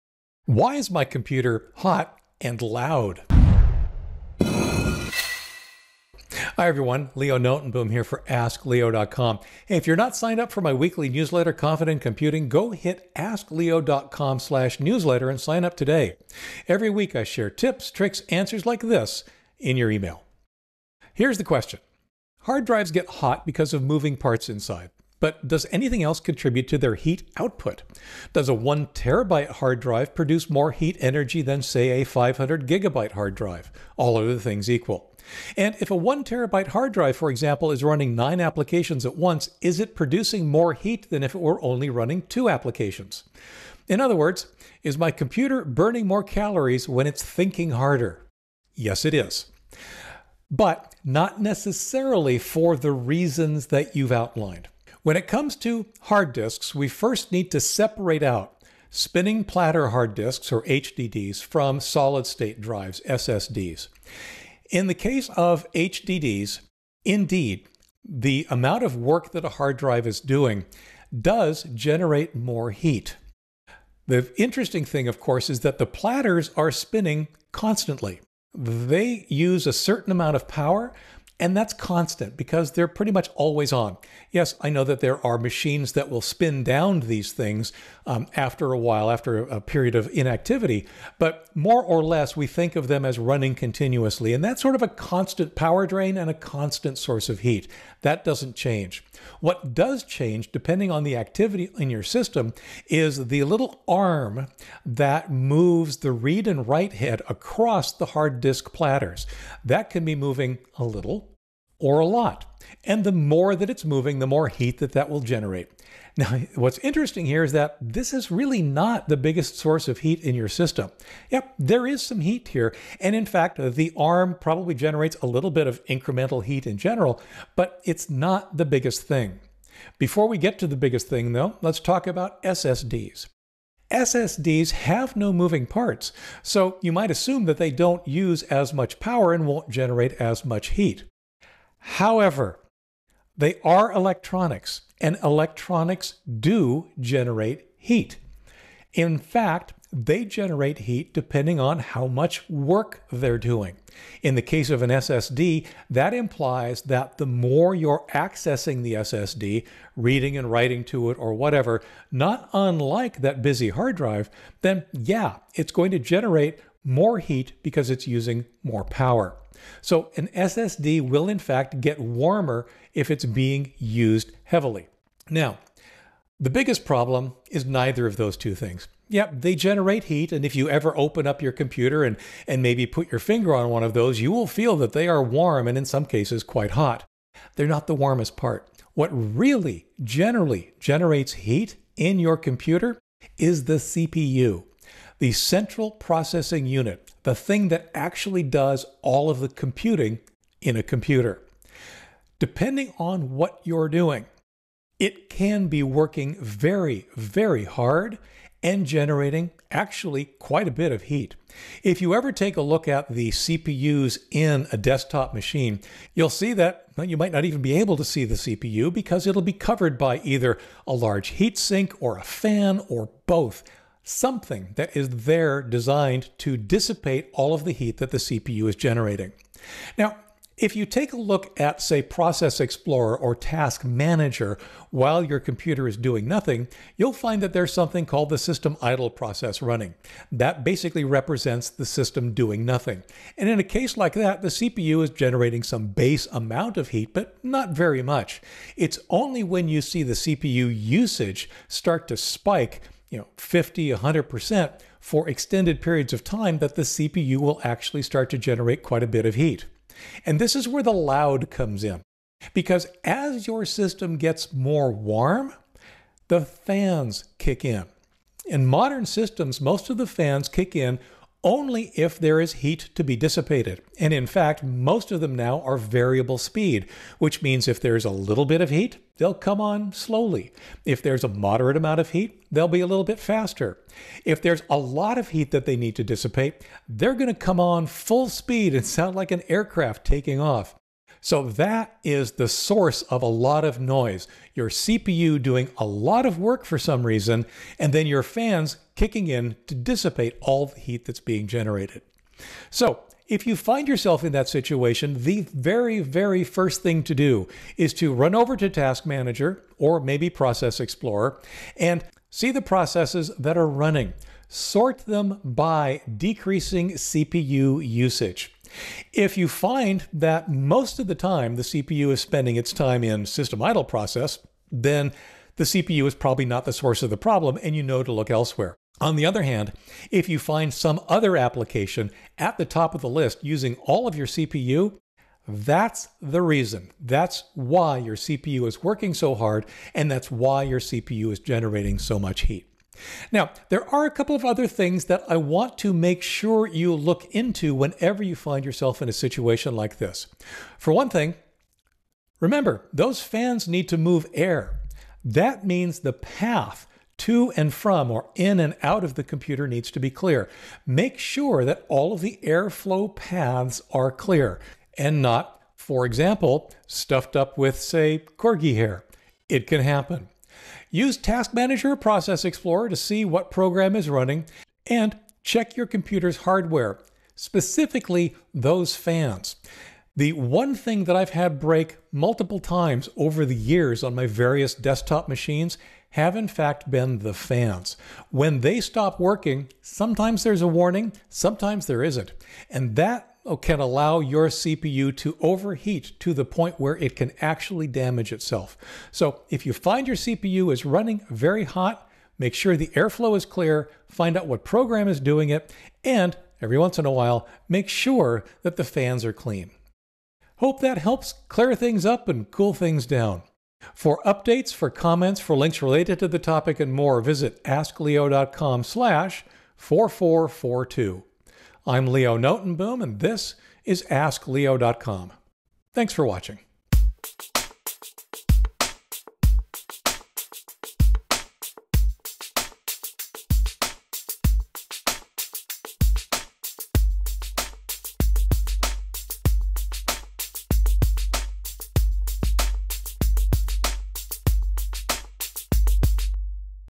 Why Is My Computer Hot and Loud?
☀️ Several components in your computer use power and generate heat in proportion to how they're used. I'll review the two biggies.

☀️ Hot and loud computers
Computer components generate more heat the more intensely they’re used. If your computer is working hard on something, be it CPU- or disk-based, that can result in more heat generated and the cooling system kicking in. Sometimes the fans used by those systems are loud when running at full speed.

Using Task Manager  to Track Down CPU Hogs ▶ idMWDDBPGCs

Chapters
0:00 Computer Hot and Loud
1:00 HDDs and power
2:10 SSDs and power
3:05 Capacity and power
3:40 The CPU and Power
4:20 Loud
5:50 Check your air flow